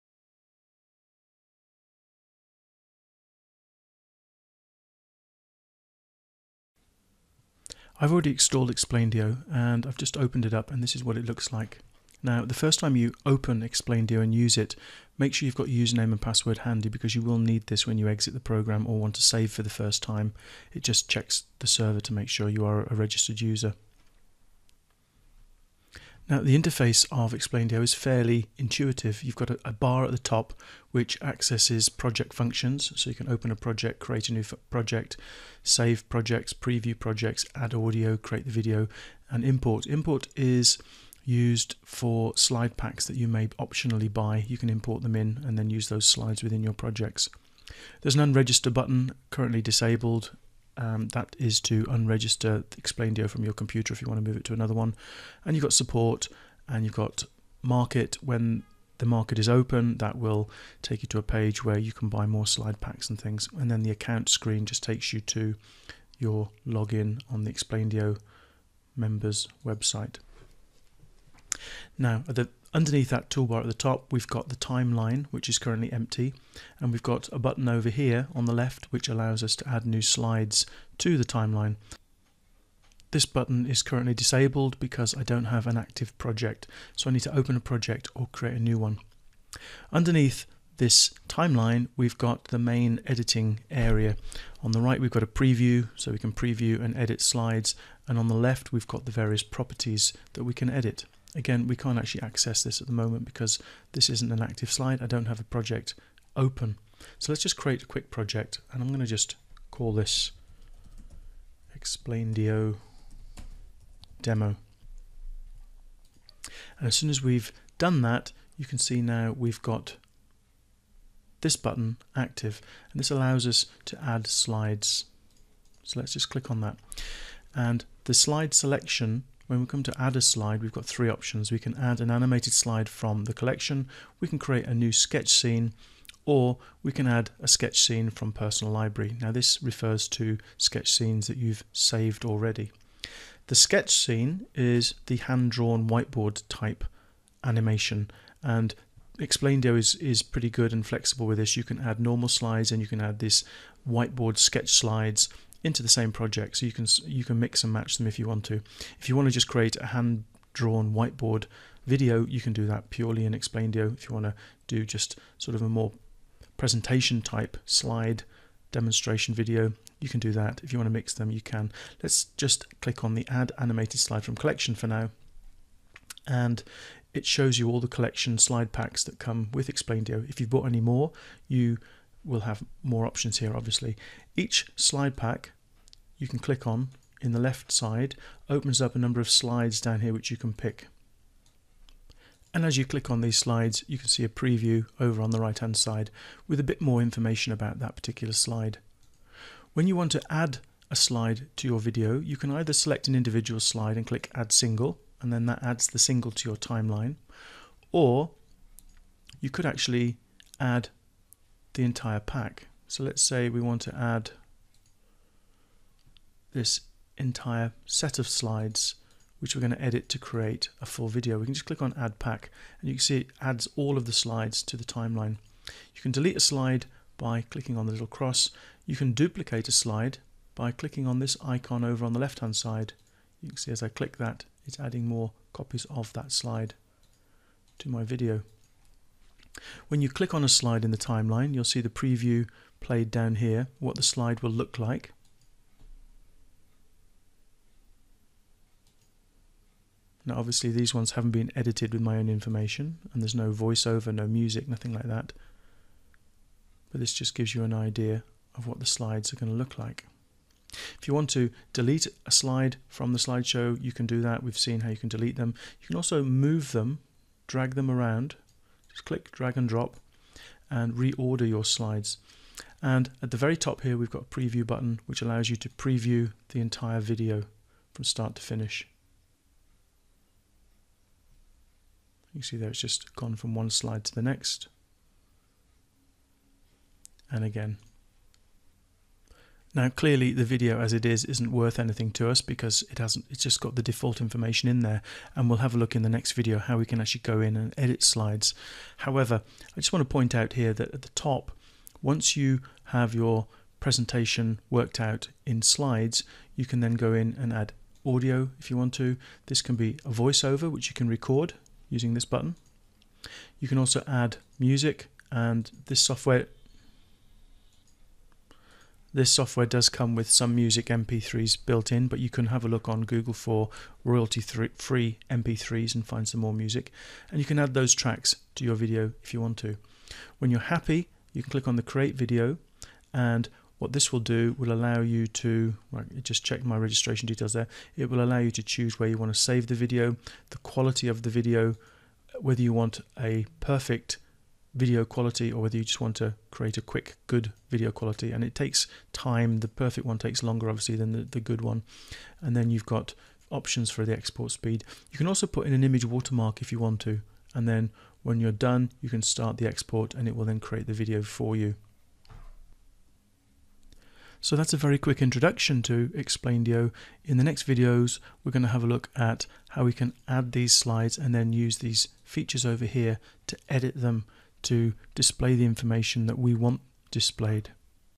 Explaindio Tutorial - Getting Started with Explaindio
Explaindio Tutorial - Explaindio is a video creation tool that is intuitive, versatile and easy to use.

If you create videos, you should check out Explaindio.